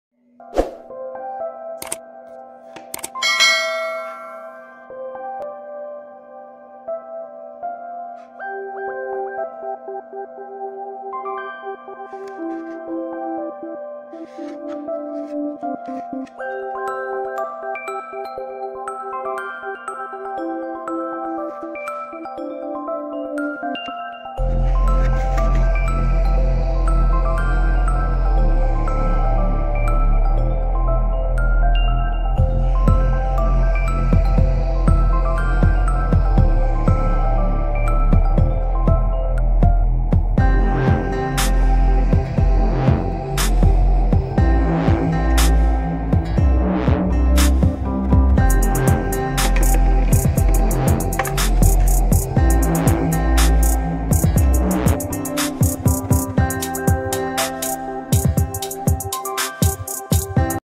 نصيحه مهمه لمحبي النباتات🌱
ضروري من وجود فتحات في اصص النباتات